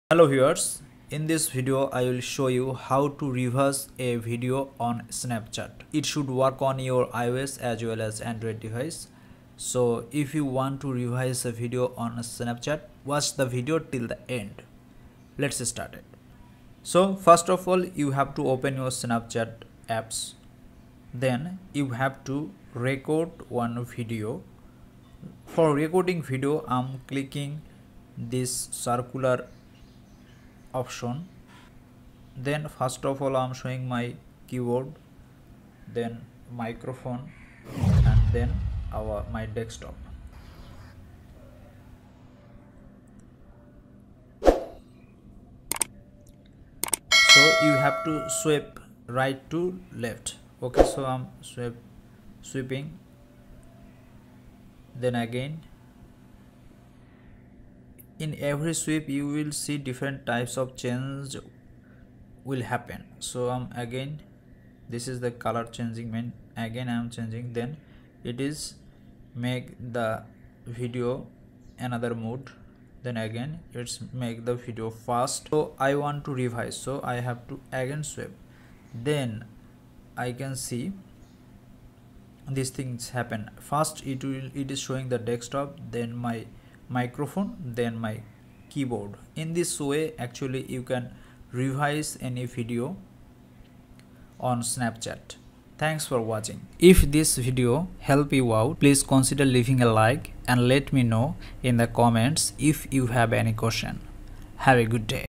How to Reverse a Video on Snapchat! (Snapchat Reverse Trick)
How to Reverse a Video on Snapchat
Want to reverse on snapchat? This is how.

Alright let's show you how you can reverse a video on snapchat:

1) Open snapchat
2) Record a video
3) Swipe to right to select filters
4) Find reverse filter

Your video is now in reverse on snapchat, well done!